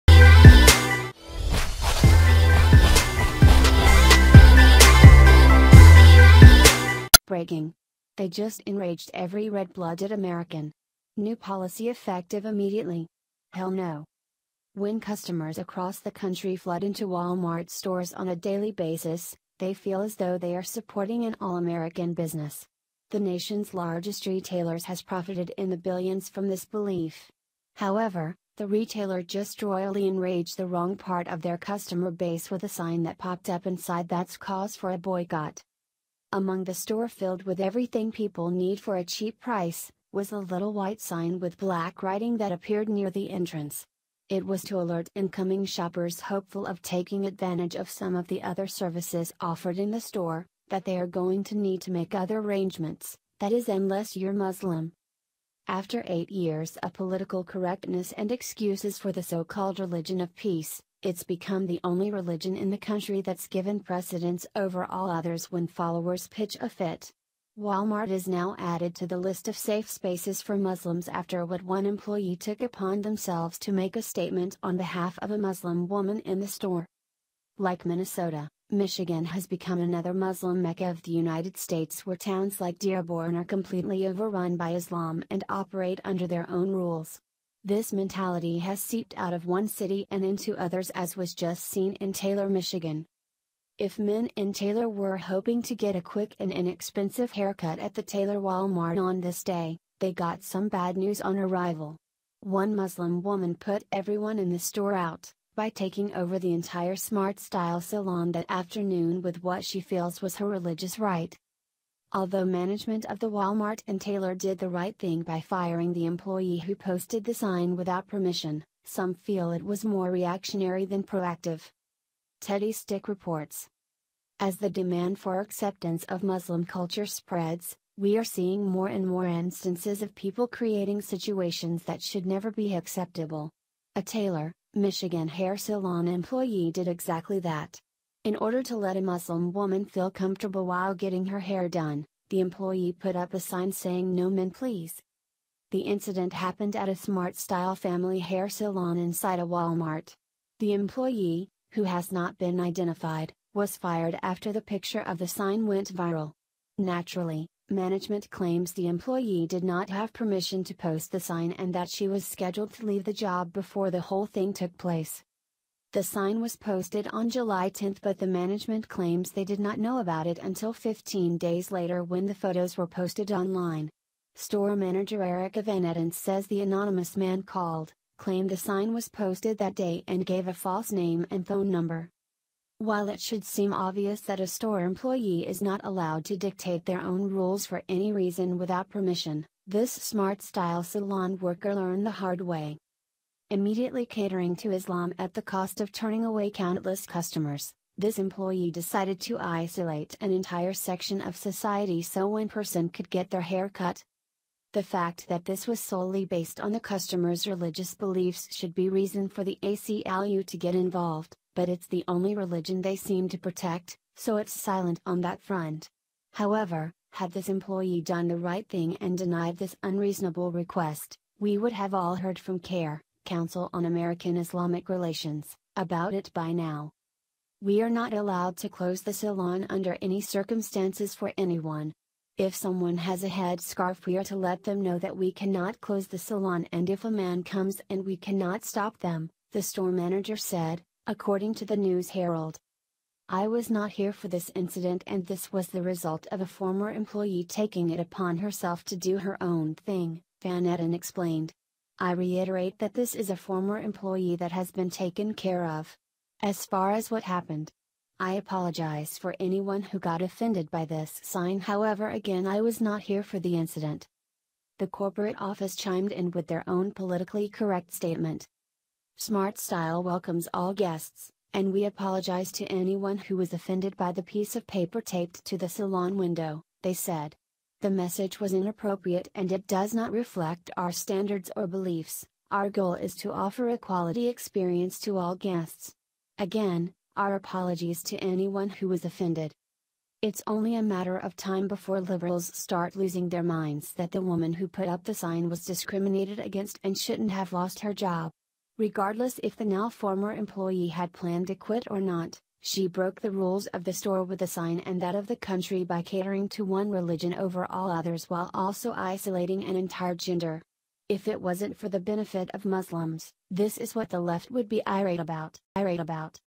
BREAKING!! They Just Enraged Every Red-Blooded American!! New Policy Effective Immediately!!
BREAKING!! They Just Enraged Every Red-Blooded American!! New Policy Effective Immediately!! HELL NO!

When customers across the country flood into Walmart stores on a daily basis, they feel as though they are supporting an all-American business. The nation’s largest retailer has profited in the billions from this belief. However, the retailer just royally enraged the wrong part of their customer base with a sign that popped up inside that’s cause for a boycott.

Among the store filled with everything people need for a cheap price, was a little white sign with black writing that appeared near the entrance. It was to alert incoming shoppers hopeful of taking advantage of some of the other services offered in the store, that they are going to need to make other arrangements — that is unless you’re Muslim.

After eight years of political correctness and excuses for the so-called “religion of peace,” it’s become the only religion in the country that’s given precedence over all others when followers pitch a fit. Walmart is now added to the list of safe spaces for Muslims after what one employee took upon themselves to make a statement on behalf of a Muslim woman in the store.

Like Minnesota, Michigan has become another Muslim “Mecca” of the United States where towns like Dearborn are completely overrun by Islam and operate under their own rules. This mentality has seeped out of one city and into others as was just seen in Taylor, Michigan.

If men in Taylor were hoping to get a quick and inexpensive haircut at the Taylor Walmart on this day, they got some bad news on arrival. One Muslim woman put everyone in the store out, by taking over the entire SmartStyle salon that afternoon with what she feels was her religious right.
Although management of the Walmart in Taylor did the right thing by firing the employee who posted the sign without permission, some feel it was more reactionary than proactive.

Teddy Stick reports:

As the demand for acceptance of Muslim culture spreads, we are seeing more and more instances of people creating situations that should never be acceptable. A Taylor, Michigan hair salon employee did exactly that. In order to let a Muslim woman feel “comfortable” while getting her hair done, the employee put up a sign saying “no men please.”

The incident happened at a SmartStyle Family Hair Salon inside a Wal-Mart. The employee, who has not been identified, was fired after the picture of the sign went viral. Naturally, management claims the employee did not have permission to post the sign and that she was scheduled to leave the job before the whole thing took place.

The sign was posted on July 10 but the management claims they did not know about it until 15 days later when the photos were posted online. Store manager Erika Van Etten says the anonymous man called, claimed the sign was posted that day and gave a false name and phone number.
While it should seem obvious that a store employee is not allowed to dictate their own rules for any reason without permission, this SmartStyle salon worker learned the hard way.

Immediately catering to Islam at the cost of turning away countless customers, this employee decided to isolate an entire section of society so one person could get their hair cut.

The fact that this was solely based on the customer’s religious beliefs should be reason for the ACLU to get involved, but it’s the only religion they seem to protect, so it’s silent on that front. However, had this employee done the right thing and denied this unreasonable request, we would have all heard from CAIR (Council on American-Islamic Relations) about it by now.

“We are not allowed to close the salon under any circumstances for anyone. If someone has a head scarf we are to let them know that we cannot close the salon and if a man comes in we cannot stop them,” the store manager said, according to the News-Herald.

“I was not here for this incident and this was the result of a former employee taking it upon herself to do her own thing, Van Etten explained. “I reiterate that this is a former employee that has been taken care of. As far as what happened. I apologize for anyone who got offended by this sign however again I was not here for the incident.”

The corporate office chimed in with their own politically correct statement:

“SmartStyle welcomes all guests, and we apologize to anyone who was offended by the piece of paper taped to the salon window,” they said. “The message was inappropriate and it does not reflect our standards or beliefs; our goal is to offer a quality experience to all guests. Again, our apologies to anyone who was offended.”